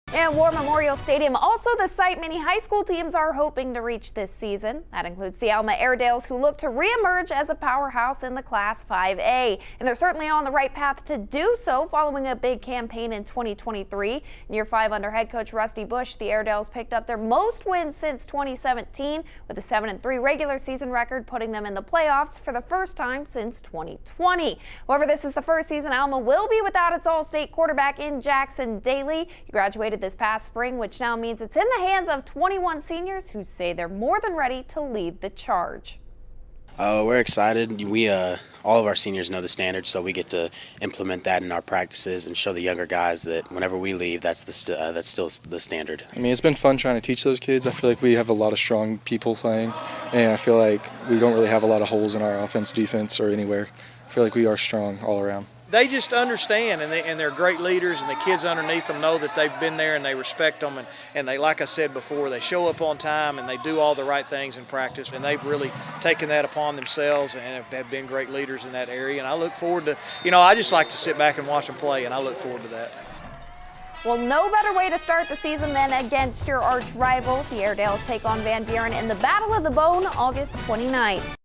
Friday Frenzy Preseason Roundup: Alma
The Airedales eye a return to the 5A state playoffs.